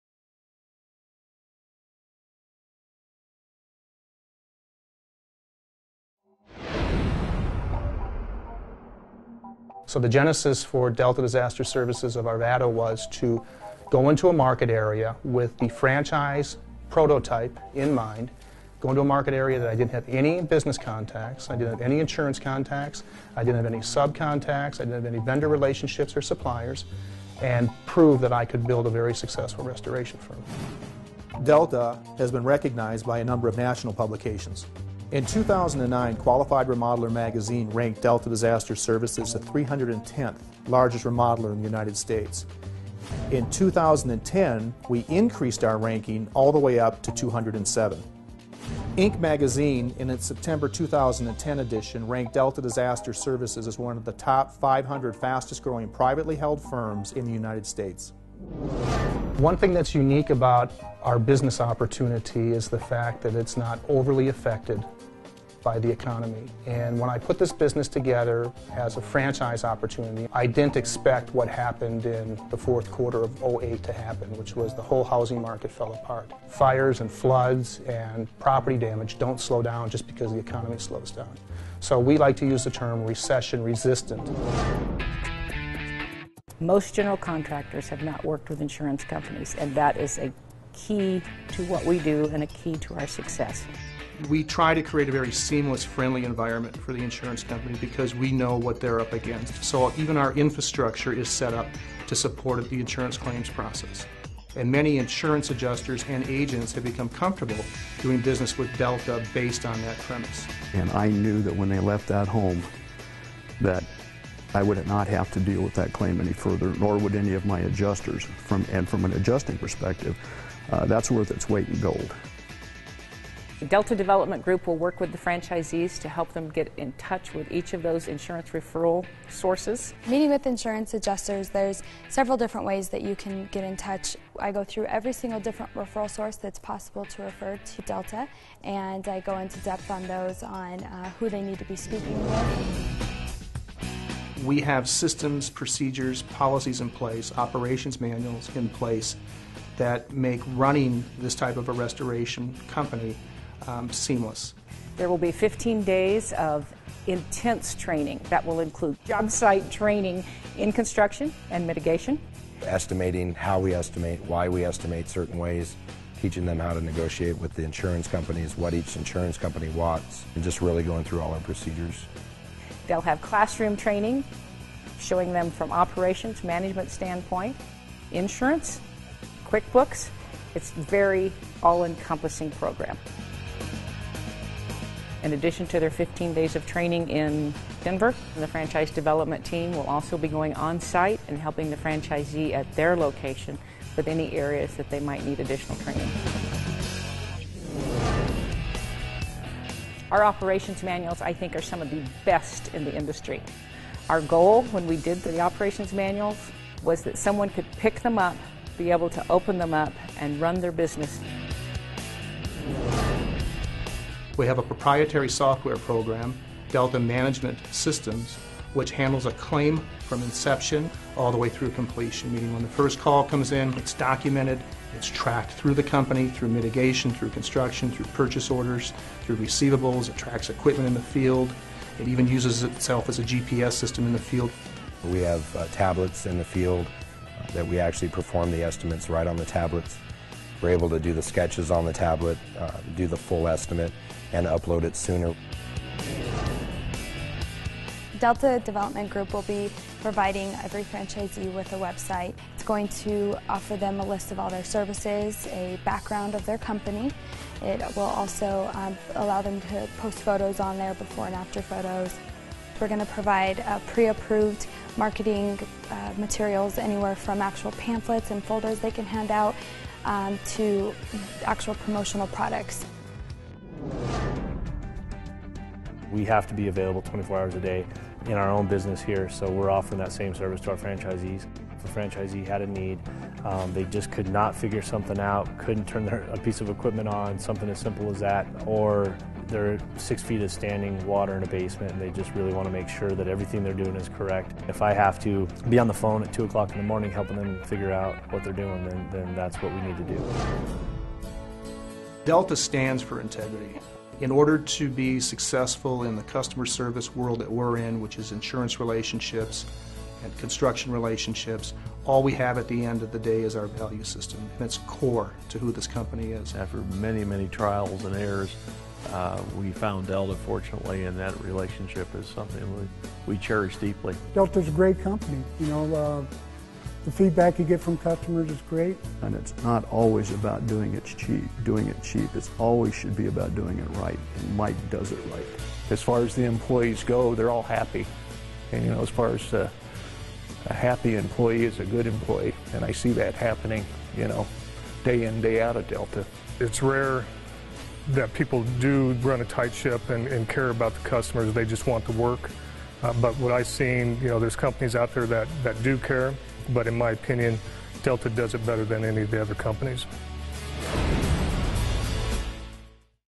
Delta Disaster Services Franchise Opportunity
Delta Disaster Services is a unique franchise that allows builders like you to bridge success in construction with the recession-resistant world of insurance restoration. Our franchises offer a one-stop source to property owners for all of their insurance claims needs–from initial emergency services to complete reconstruction.